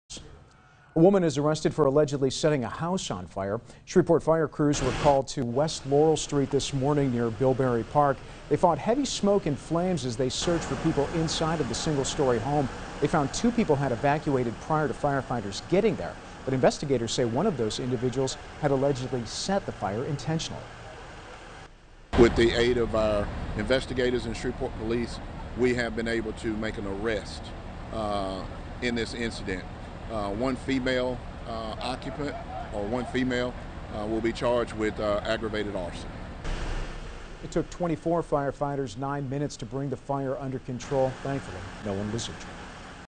Woman charged with arson in Queensborough house fire
Police have arrested a woman in connection with a fire that damaged a home near Bilberry Park in Shreveport‘s Queensborough neighborhood Thursday morning.